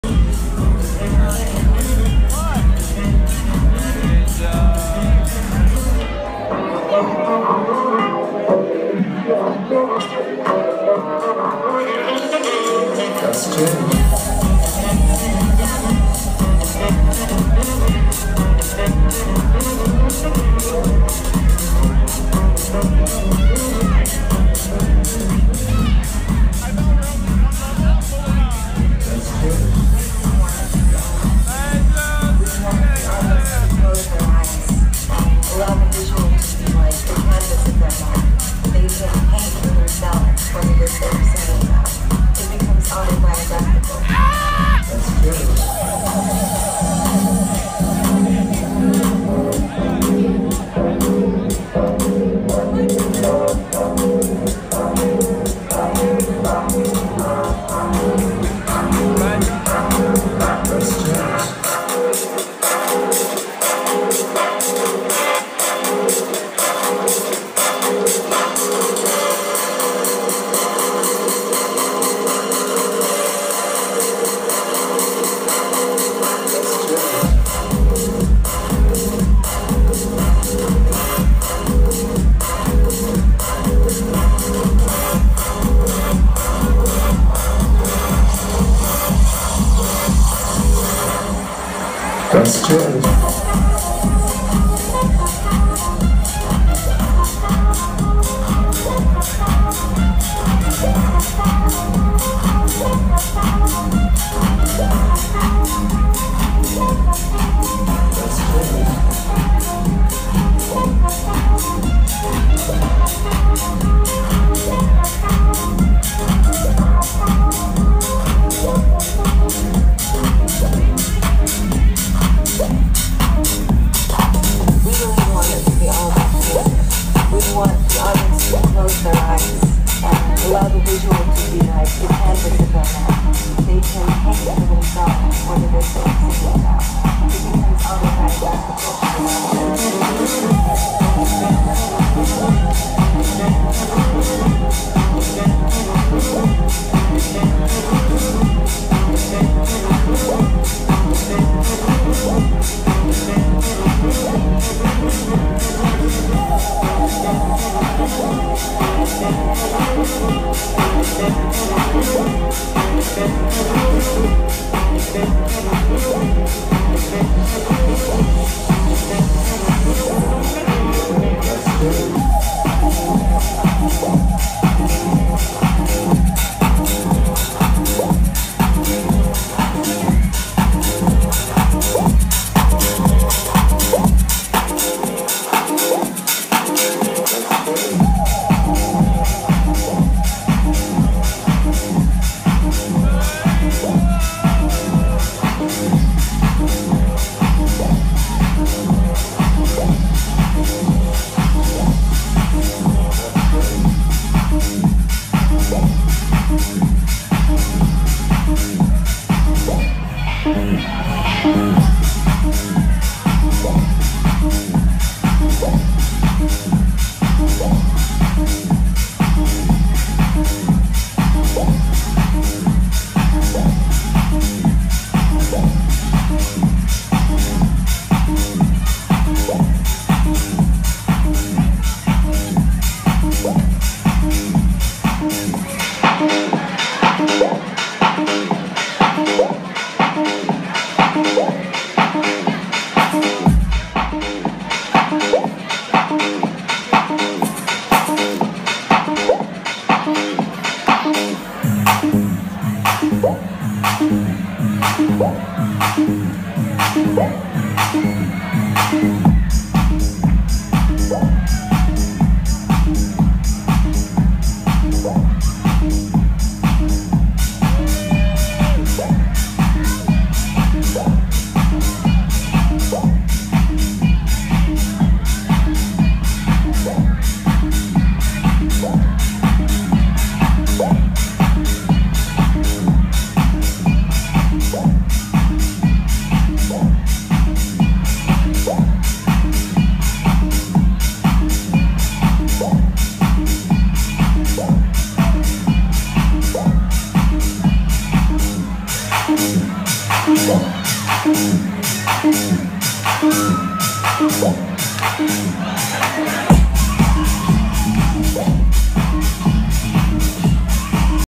Marc farina,playing that FUNK.garden theater,Detroit 2015.chea
Mark farina(house music LEGEND)playing that DANKY dank tunes!easter weekend at the brand new garden theater,Detroit Michigan,2015. sweeeeeeet!